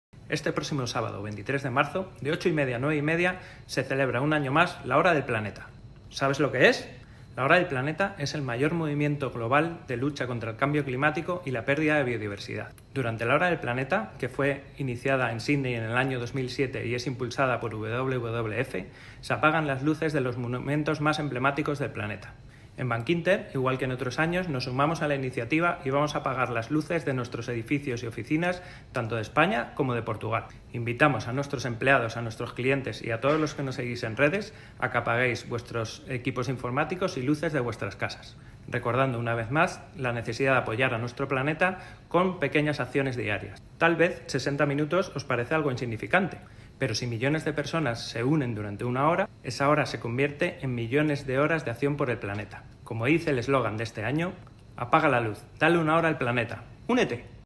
¿Sabes lo que es “La Hora del Planeta”?
La Hora del Planeta es el mayor movimiento global en favor de la naturaleza, símbolo de la lucha contra el cambio climático y la pérdida de biodiversidad. José Antonio Barberá, responsable del departamento de Sostenibilidad en Bankinter, te cuenta todos los detalles.